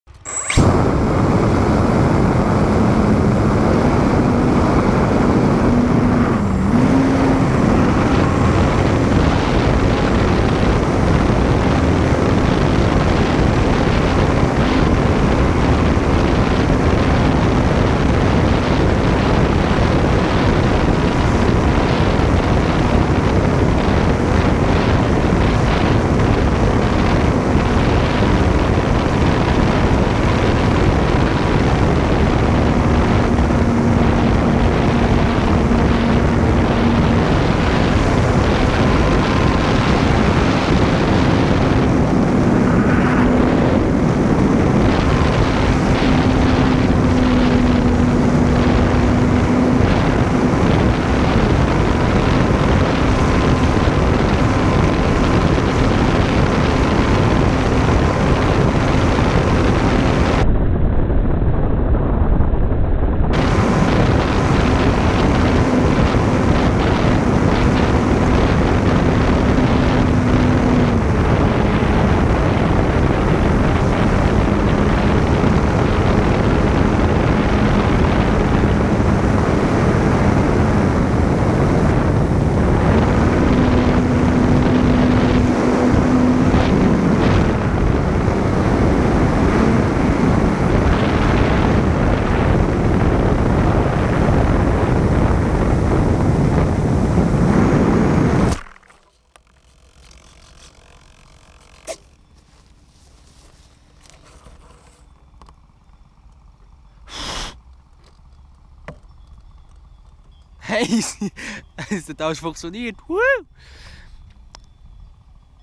Awesome GoPro on FT Tiny Trainer
I biuld the FT Tiny Trainer and i love it. So one dasy i decided to put a GoPro on my plane and those are the best moments...

Its very hard to fly it, because of the extra weight of the actioncamera.